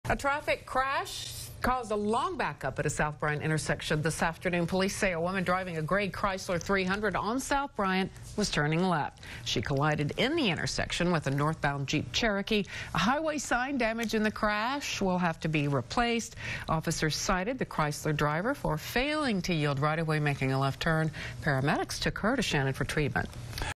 One hospitalized in S. Bryant crash
One person was hospitalized following a crash near the intersection of South Bryant and Avenue L on Thursday.